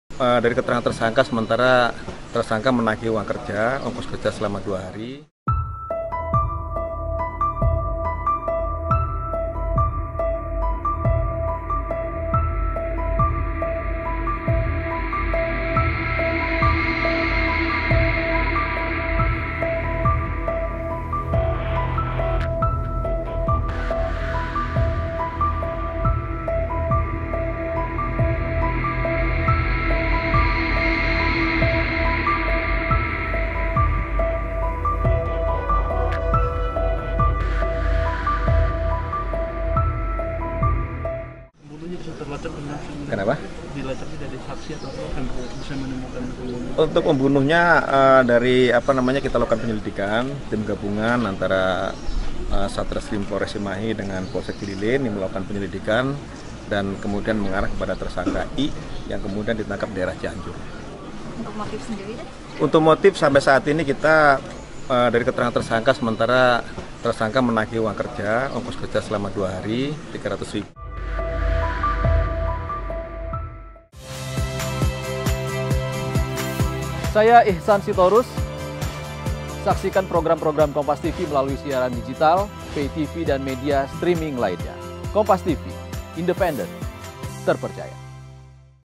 Geger! Mayat Dikubur dalam Rumah di Bandung, Polisi Beberkan Motif Pelaku
KOMPAS.TV-Geger! Mayat Dikubur dalam Rumah di Bandung, Polisi Beberkan Motif Pelaku